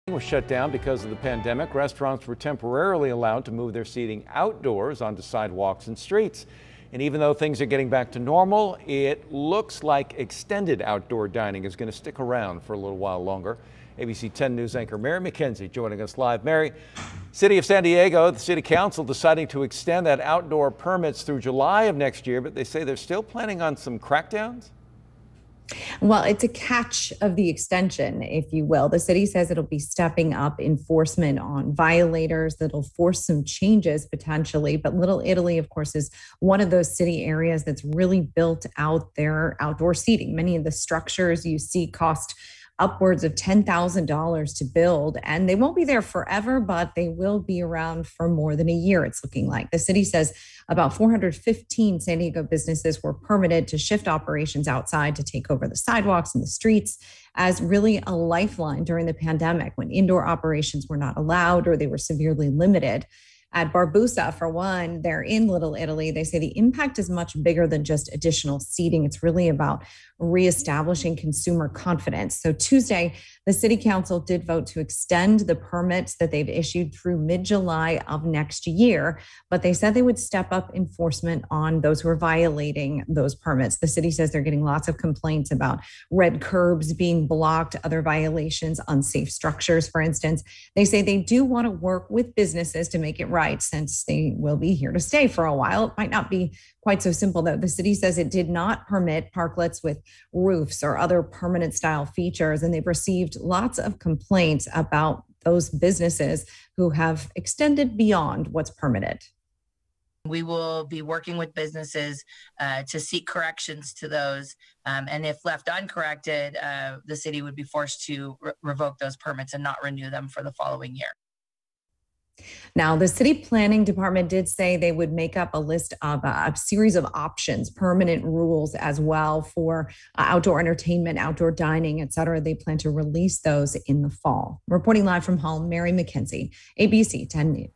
City of San Diego extends outdoor dining for a year
The City of San Diego will extend outdoor dining for another year, but some adjustments to the rules are being made.